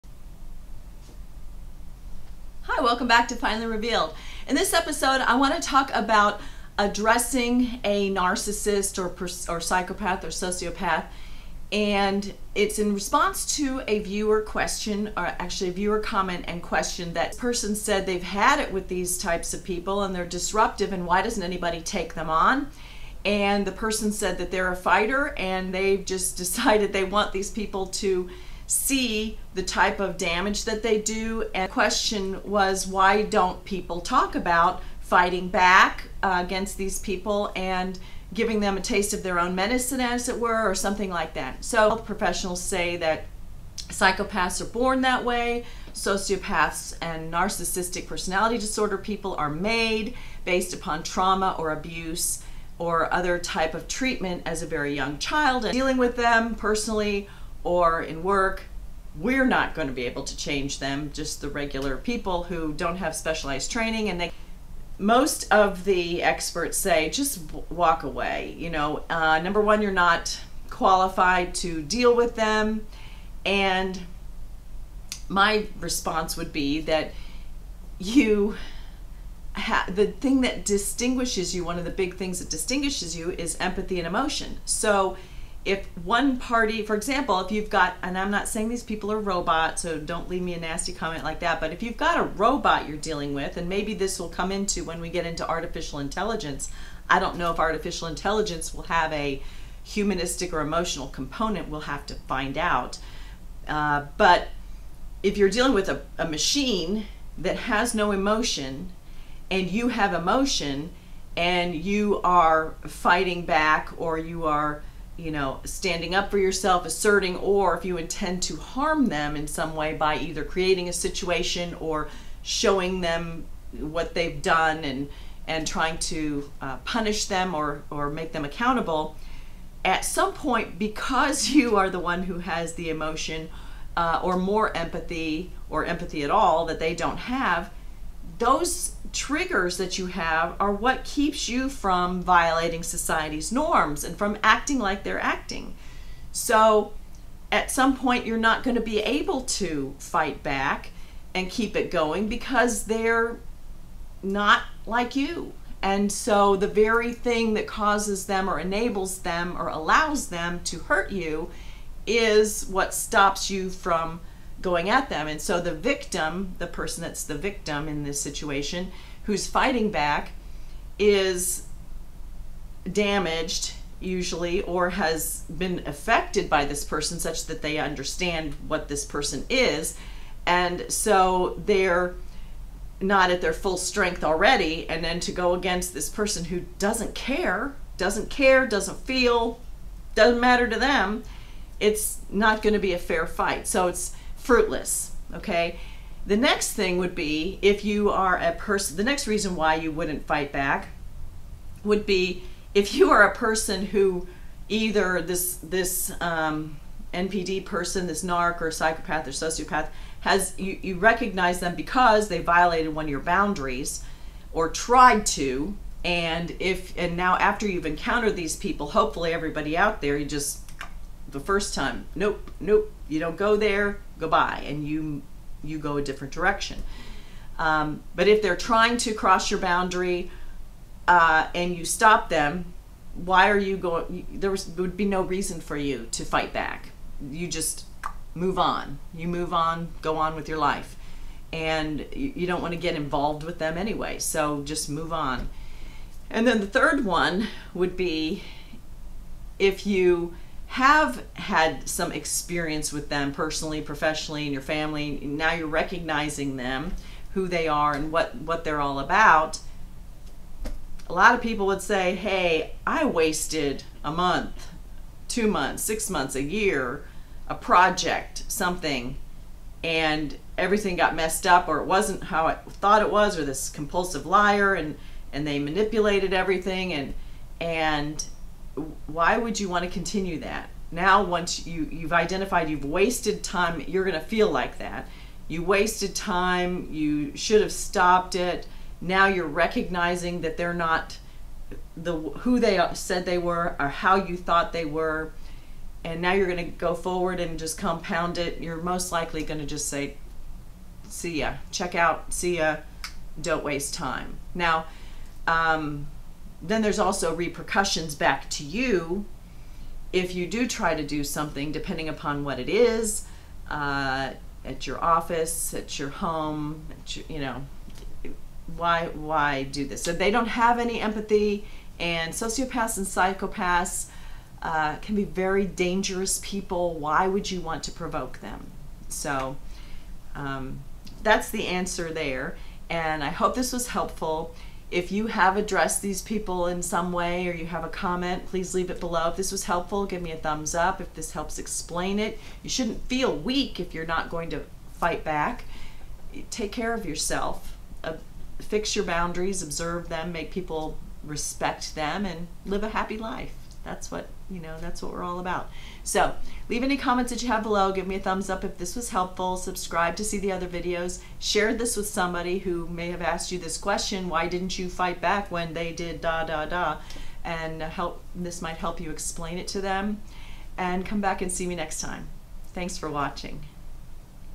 Confronting the narcissist
Answering a viewer comment and question about why we don't include confronting or fighting back against the narc.